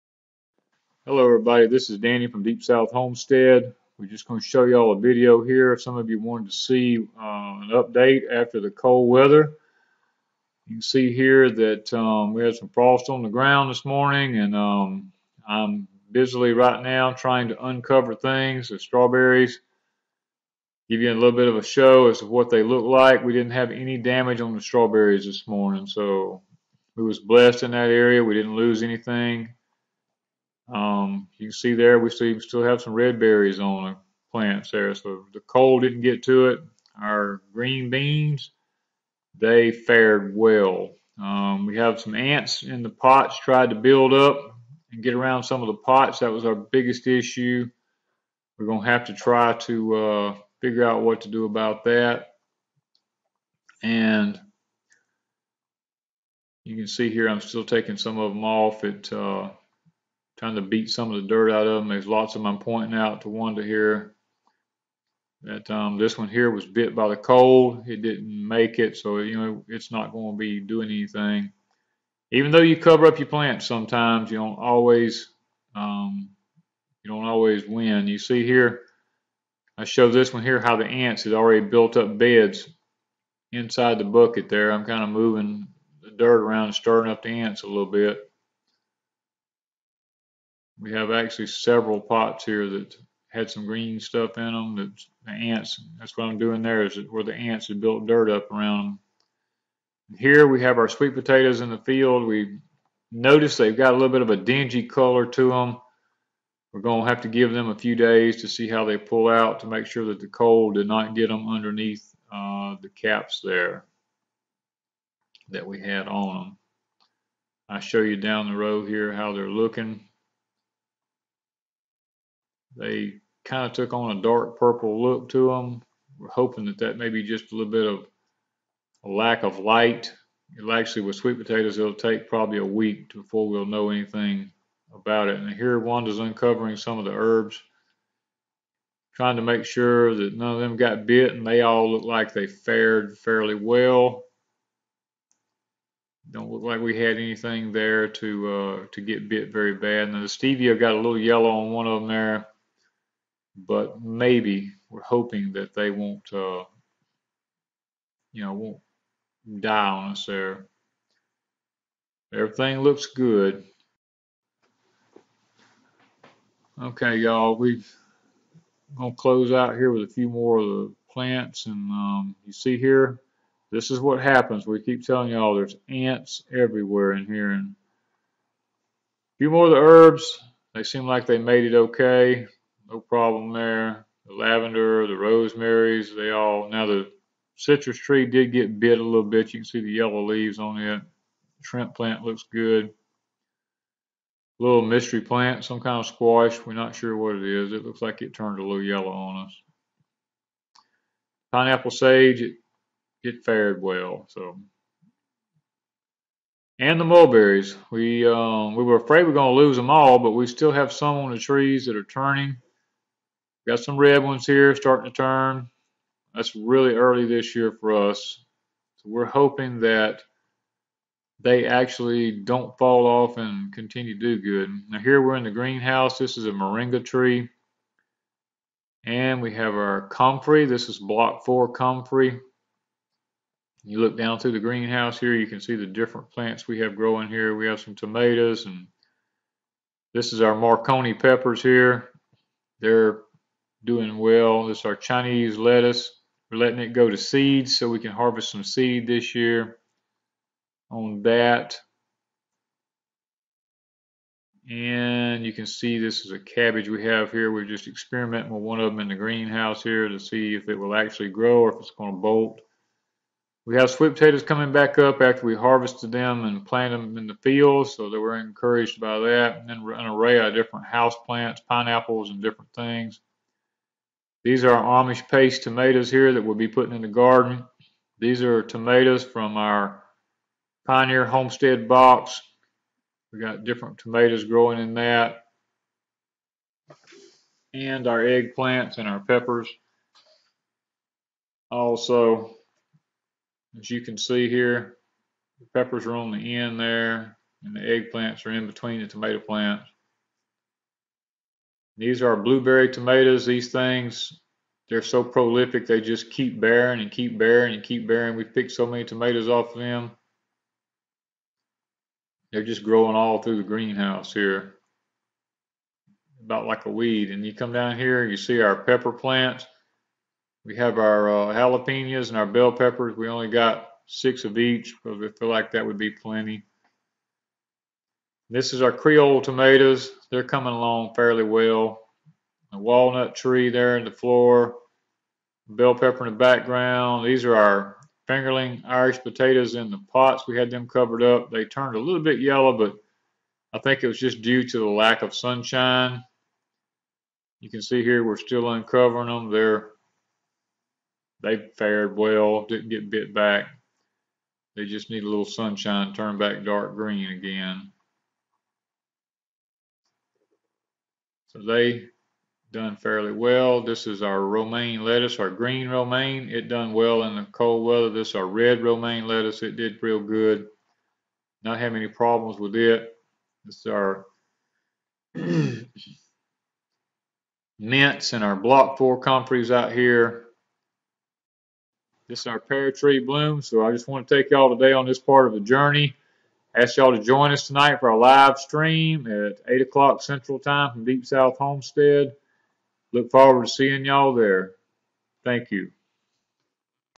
What Did the Freeze Do to our Plants?
What did the freeze do to our plants?  We take you on a walk while we uncover the plants after a couple of days of near freezing tempts at night.

Cooking Southern Style with Deep South Homestead cookbook Sweet Potato Manuel to order